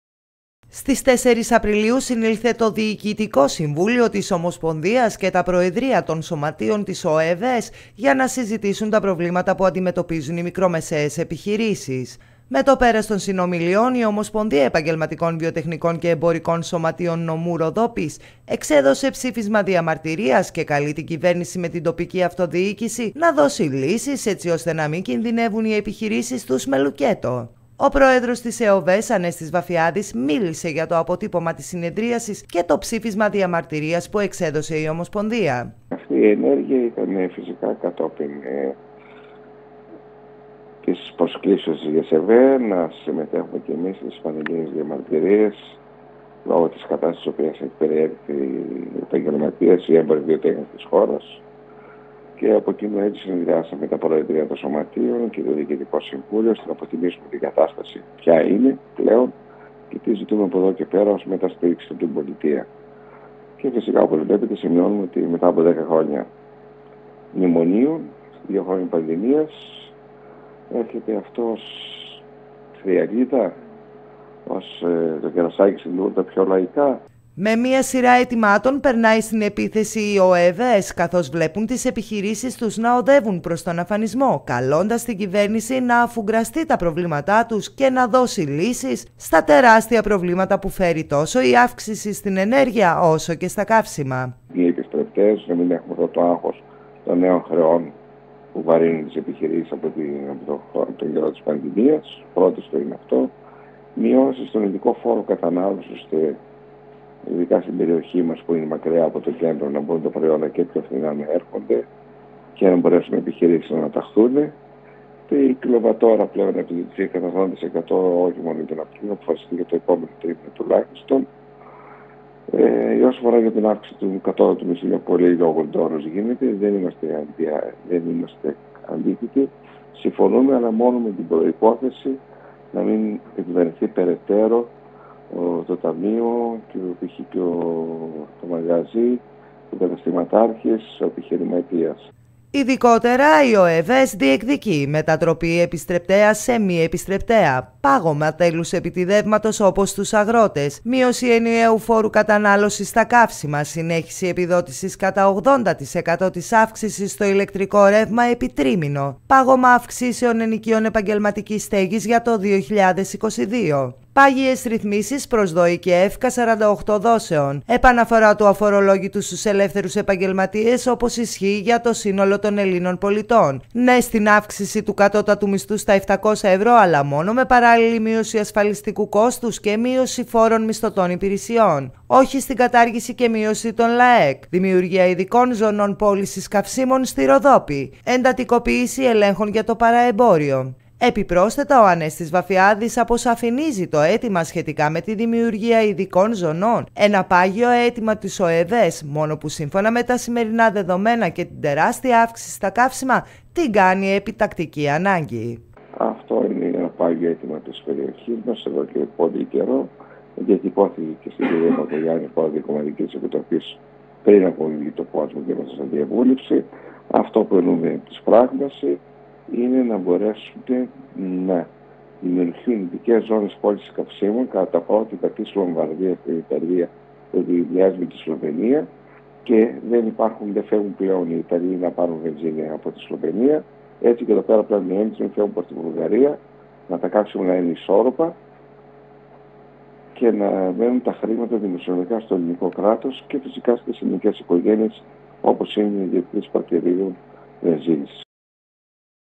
Ειδικές ζώνες πώλησης καυσίμων στη Ροδόπη ζητάει η ΟΕΒΕΣ
Ψήφισμα διαμαρτυρίας εξέδωσε η Ομοσπονδία Επαγγελματικών Βιοτεχνικών και Εμπορικών Σωματίων Νομού Ροδόπης  στις 4 Απριλίου , μετά την συνεδρίαση που πραγματοποιήθηκε με θέμα τα προβλήματα που απασχολούν τον χώρο των μικρομεσαίων επιχειρήσεων. Με μία σειρά αιτημάτων περνάει στην επίθεση η ΟΕΒΕΣ  καθώς βλέπουν τις επιχειρήσεις τους να οδεύουν προς τον αφανισμό καλώντας την κυβέρνηση να αφουγκραστεί τα προβλήματά τους και να δώσει λύσεις στα τεράστια προβλήματα που φέρει τόσο η αύξηση στην ενέργεια όσο και στα καύσιμα